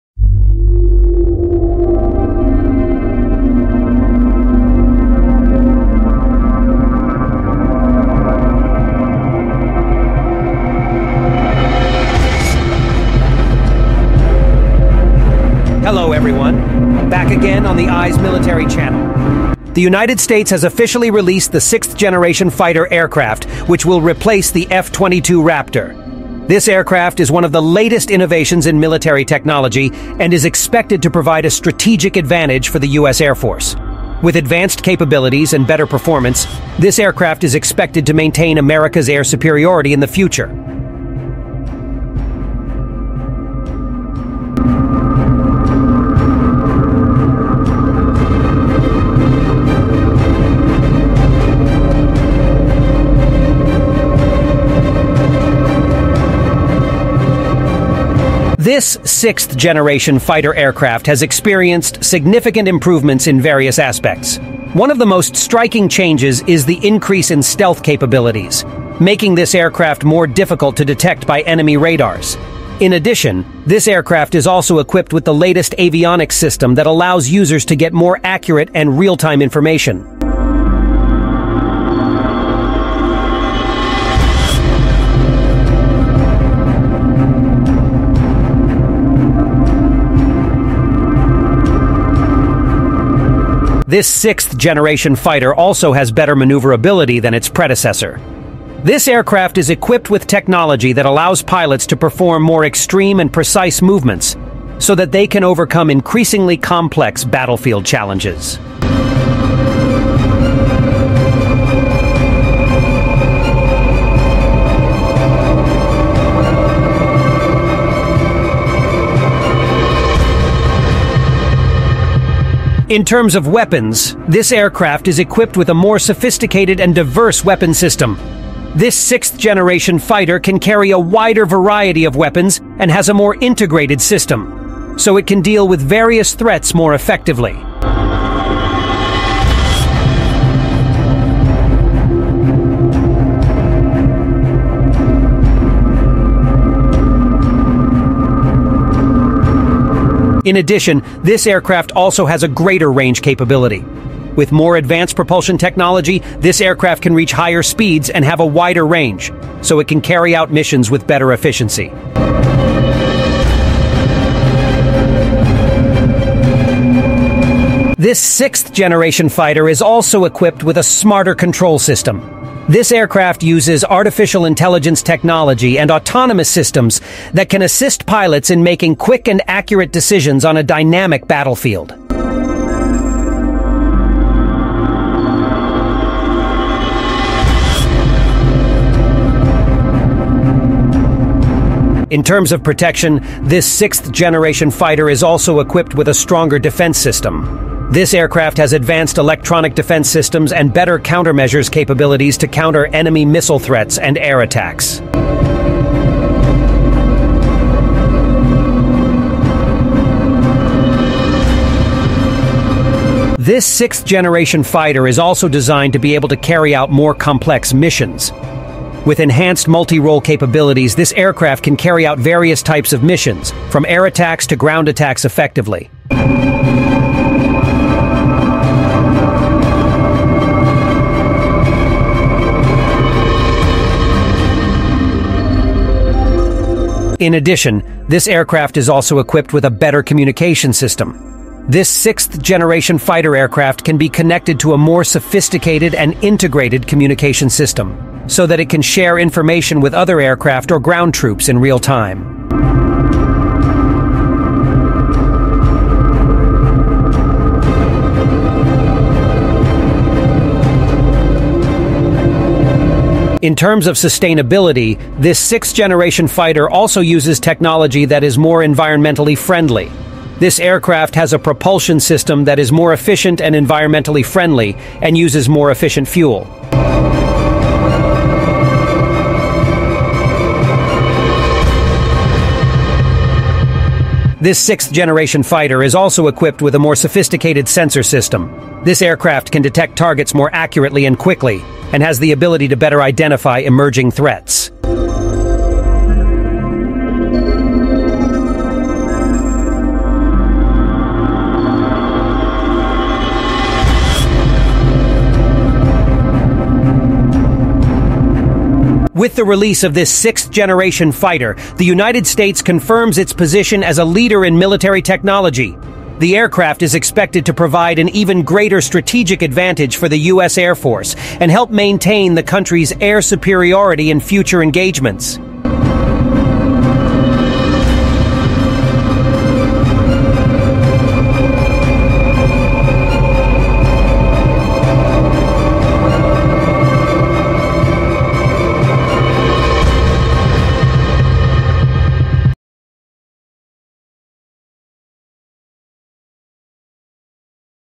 Finally! A $155.4 billion F-22 Raptor replacement fighter jet is ready for development
The United States has officially released the 6th generation fighter that will replace the F-22 Raptor. This aircraft is one of the latest innovations in military technology and is expected to provide a strategic advantage for the US Air Force. With advanced capabilities and better performance, this aircraft is expected to maintain America's air superiority in the future.